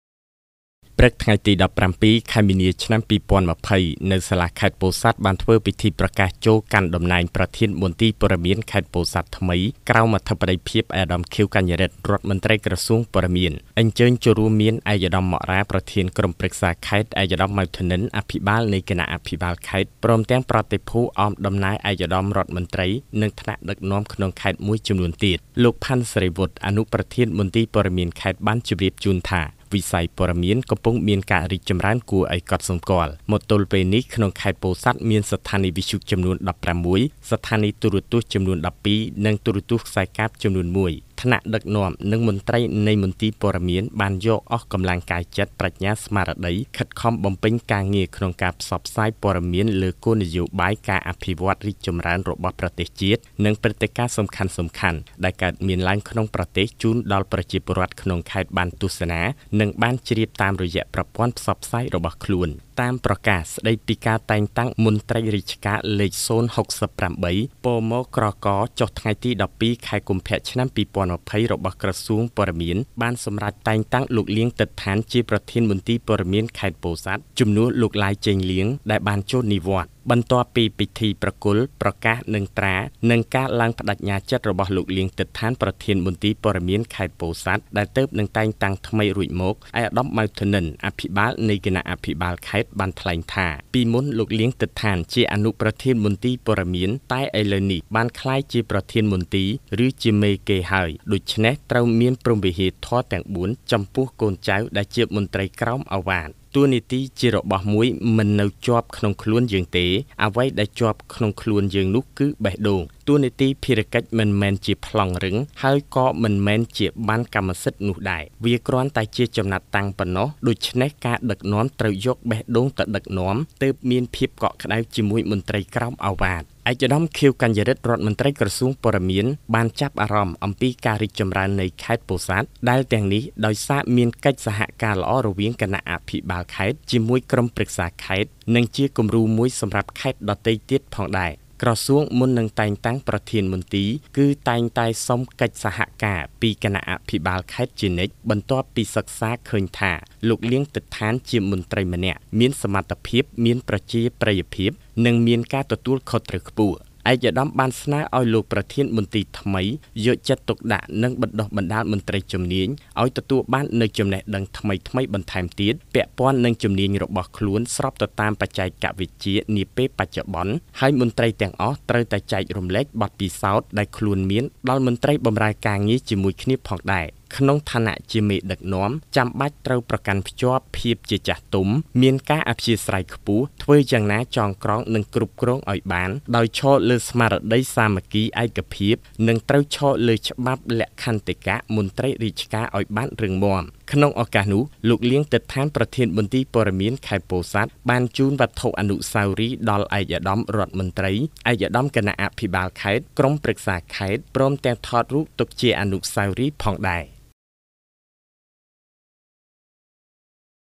ពិធីប្រកាសតែងតាំងប្រធានមន្ទីរព័ត៌មានខេត្តពោធិ៍សាត់ថ្មី ជំនួសប្រធានមន្ទីរចាស់ដែលចូលនិវត្ត
ខេត្តពោធិ៍សាត់៖ព្រឹកថ្ងៃទី១៧ខែមិនាឆ្នាំ២០២០ នៅសាលាខេត្តពោធិ៍សាត់ មានធ្វើពិធីប្រកាសចូលកាន់តំណែងប្រធានមន្ទីរព័ត៌មានខេត្តពោធិ៍សាត់ថ្មី ក្រោមអធិបតីភាពឯកឧត្តម ខៀវ កាញារីទ្ធ រដ្ឋមន្ត្រីក្រសួងព័ត៌។អញ្ជើញចូលរួមមានឯកឧត្តម ម៉ក់ រ៉ា ប្រធានក្រុមប្រឹក្សាខេត្ត ឯកឧត្តម ម៉ៅ ធនិន អភិបាលនៃគណៈអភិបាលខេត្ត ព្រមទាំងប្រតិភូអមដំណើរឯកឧត្តមរដ្ឋមន្ត្រី និងថ្នាក់ដឹកនាំក្នុងខេត្តមួយចំនួនទៀត។
 លោក ផាន់ សេរីវុធ អនុប្រធានមន្ទីរព័ត៌មានខេត្តបានជម្រាបជូនថា វិស័យព័ត៌មានកំពុងមានការរីកចំរើនគួរឲ្យកត់សម្គាល់ មកទល់ពេលនេះក្នុងខេត្តពោធិ៍សាត់មានស្ថានីយ៍វិទ្យុចំនួន១៦ ស្ថានីយ៍ទូរទស្សន៍ចំនួន១២ និងទូរទស្សន៍ខ្សែកាបចំនួនមួយ។ ថ្នាក់ដឹកនាំនិងមន្ត្រីនៃមន្ទីរព័ត៌មាន បានយកអស់កម្លាំងកាយចិត្ត ប្រាជ្ញា ស្មារតី ខិតខំបំពេញការងារក្នុងការផ្សព្វផ្សាយព័ត៌មានលើគោលនយោបាយ ការអភិវឌ្ឍន៍រីកចំរើនរបស់ប្រទេសជាតិ និងព្រឹត្តិការណ៍សំខាន់ៗដែលកើតមានឡើងក្នុងប្រទេស ជូនដល់ប្រជាពលរដ្ឋក្នុងខេត្តបានទស្សនានិងបានជ្រាបតាមរយៈប្រព័ន្ធផ្សព្វផ្សាយរបស់ខ្លួន។
 តាមប្រកាសស្ដីពីការតែងតាំងមន្ត្រីរាជការលេខ ០៦៨ ពម.ប្រក ចុះថ្ងៃទី១២ខែកុម្ភះឆ្នាំ២០២០ របស់ក្រសួងព័ត៌មានបានសម្រេចតែងតាំងលោក លាង ទិតដ្ឋាន ជាប្រធានមន្ទីរព័ត៌មានខេត្តពោធិ៍សាត់ ជំនួសលោក ឡាយ ចេងលៀង ដែលបានចូលនិវត្តន៍។
 បន្ទាប់ពីពិធីប្រគល់ប្រកាសនិងត្រា និងការឡើងប្ដេជ្ញាចិត្តរបស់លោក លាង ទិតដ្ឋាន ប្រធានមន្ទីរព័ត៌មានខេត្តពោធិ៍សាត់ដែលទើបតែងតាំងថ្មីរួចមក ឯកឧត្តម ម៉ៅ ធនិន អភិបាលនៃគណៈអភិបាលខេត្តថ្លែងថា  ពីមុនលោក លាង ទិតដ្ឋាន ជាអនុប្រធានមន្ទីរ តែឥឡូវនេះបានក្លាយជាប្រធានមន្ទីរ រឺជាមេគេហើយ ដូច្នេះត្រូវមានព្រហ្មវិហាធម៌ទាំង០៤ចំពោះកូនចៅដែលជាមន្ត្រីក្រោមឱវាទ។ តួនាទីជារបស់មួយមិននៅជាប់ក្នុងខ្លួនយើងទេ អ្វីដែលជាប់ក្នុងខ្លួននោះគឺបេះដូង។ តួនាទីភារកិច្ចមិនមែនជាប្លង់រឹងហើយក៍មិនមែនជាប័ណ្ណកម្មសិទ្ធិនោះដែរ វាគ្រាន់តែជាចំណាត់តាំងប៉ុណ្ណោះ ដូច្នេះការដឹកនាំត្រូវយកបេះដូងទៅដឹកនាំ ទើបមានភាពកក់ក្ដៅជាមួយមន្ត្រីក្រោមឱវាទ។
 ឯកឧត្តម ខៀវ កាញារីទ្ធ រដ្ឋមន្ត្រីក្រសួងព័ត៌មាន បានចាប់អារម្មណ៍អំពីការរីកចំរើននៃខេត្តពោធិ៍សាត់ ដែលទាំងនេះដោយសារមានកិច្ចសហការល្អរវាងគណៈអភិបាលខេត្តជាមួយក្រុមប្រឹក្សាខេត្ត និងជាគម្រូមួយសម្រាប់ខេត្តដទៃផងដែរ។ ក្រសួងមុននឹងតែងតាំងប្រធានមន្ទីរគឺ តែងតែសុំកិច្ចសហការពីគណៈអភិបាលខេត្តជានិច្ច បន្ទាប់ពីសិក្សាឃើញថា លោក លាង ទិតដ្ឋាន ជាមន្ត្រីម្នាក់មានសមត្ថភាព មានប្រជាប្រិយភាព និងមានការទទួលខុសត្រូវខ្ពស់។ ឯកឧត្តមស្នើឲ្យលោកប្រធានមន្ទីរថ្មី យកចិត្តទុកដាក់បណ្ដុះបណ្ដាលមន្ត្រីជំនាញ ឲ្យទទួលបាននូវចំណេះដឹងថ្មីៗបន្ថែមទៀត ពាក់ព័ន្ធនឹងជំនាញរបស់ខ្លួនស្របតាមបច្ចេកវិទ្យានាពេលបច្ចុប្បន្ន ហើយមន្ត្រីទាំងអស់ត្រូវចែករំលែកបទពិសោធន៍ដែលខ្លួនមាន ដល់មន្ត្រីបម្រើការងារជាមួយគ្នាផងដែរ។ ក្នុងឋានៈជាមេដឹកនាំ ចាំបាច់ត្រូវប្រកាន់ខ្ជាប់ភាពជាចាស់ទុំ មានការអធ្យាស្រ័យខ្ពស់ ធ្វើយ៉ាងណាចងក្រងនិងគ្រប់គ្រងឲ្យបាន ដោយឈរលើស្មារតីសាមគ្គីឯកភាព និងត្រូវឈរលើច្បាប់សហលក្ខន្តិកៈមន្ត្រីរាជការឲ្យបានរឹងមាំ។
 ក្នុងឱកាសនោះលោក លាង ទិតដ្ឋាន ប្រធានមន្ទីរព័ត៌មានខេត្តពោធិ៍សាត់ បានជូនវត្ថុអនុស្សាវរីយ៍ដល់ឯកឧត្តមរដ្ឋមន្ត្រី ឯកឧត្តមគណៈអភិបាលខេត្ត ក្រុមប្រឹក្សាខេត្ត ព្រមទាំងថតរូបទុកជាអនុស្សាវរីយ៍ផងដែរ៕ សែម សុគន្ធ   (មានរូប)